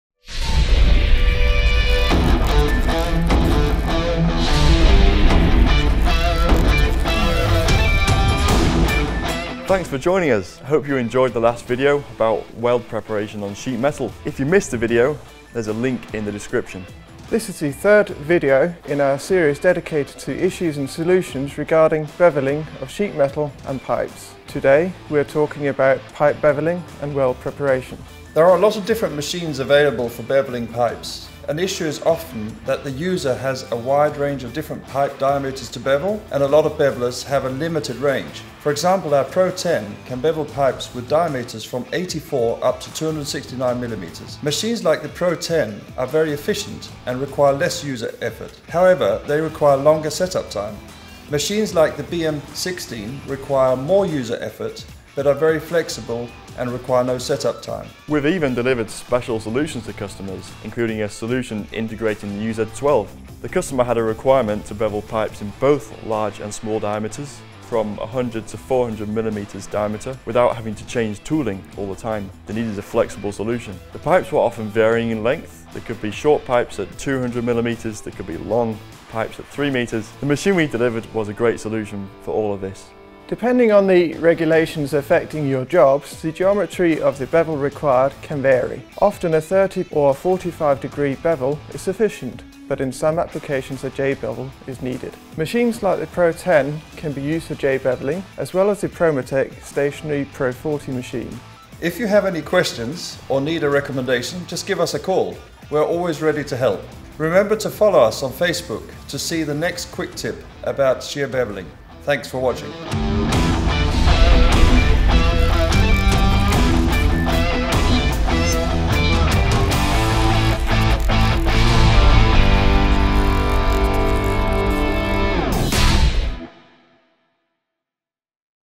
Pipe bevelling and weld preparation
Our new video deals with pipe bevelling and weld preparation.

Pipe bevelling is a crucial pre-welding process. There are a lot of machines that can be used to bevel pipes but there is often a need to work with different pipe sizes and diameters at the same time.

This is why we are presenting our range of innovative machines that can be used on a variety of different pipe diameters (up to Ø400) without having to change the tooling too often.

We are also recommending the optimal solutions for different bevelling types including 30, 45-degree and J-bevelling.